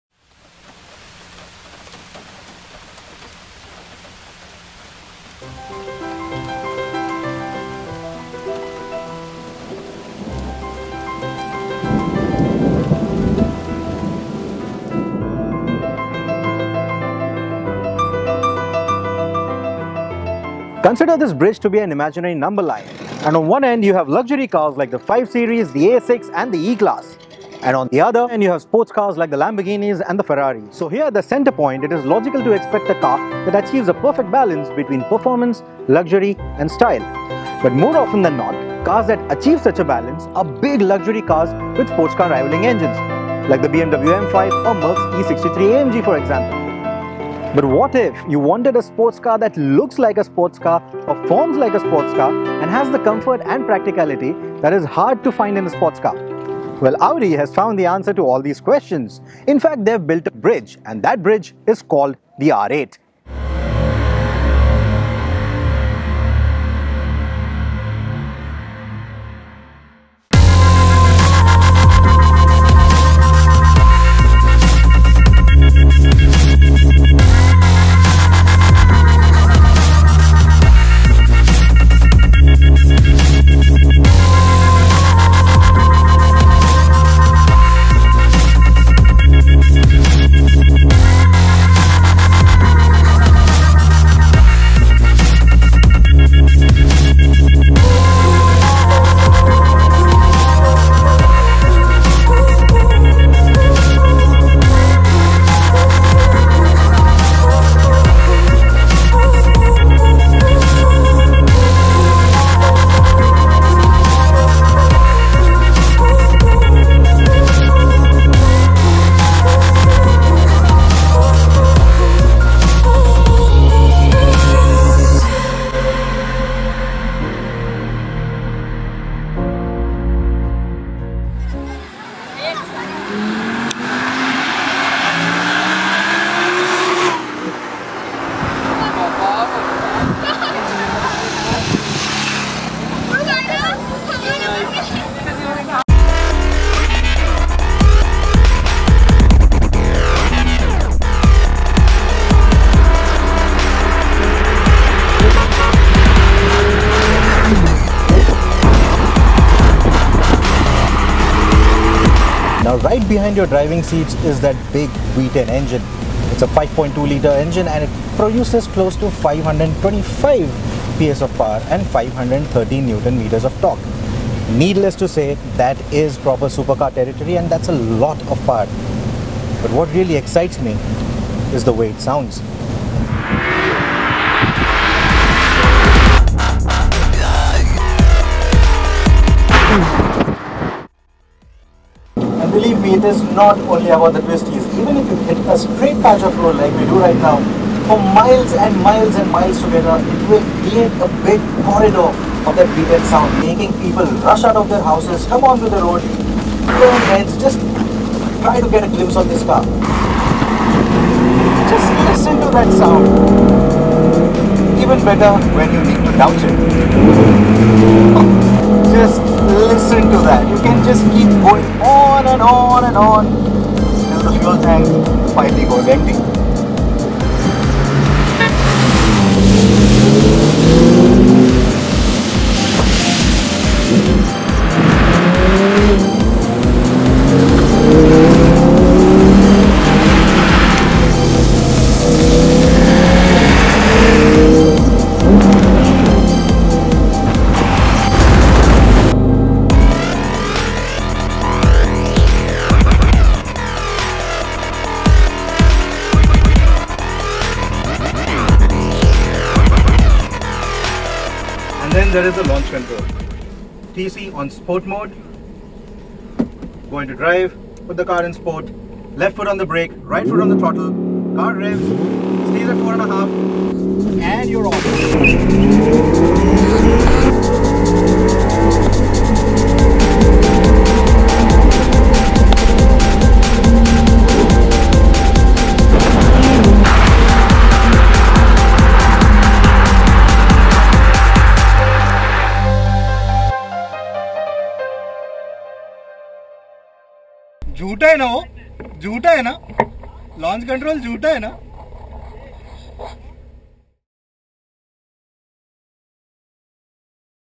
A day out with the Audi R8 V10
We have driven her on the race track and have ogled at her every time we have seen her - but one fine day we took the Audi R8 for day-long spin through some twisty mountain roads amidst rains, lush greenery, school kids and amazed villagers who were ready to cordon off the roads only to hear the beastly V10 rev to its limits. Boy what a day that was!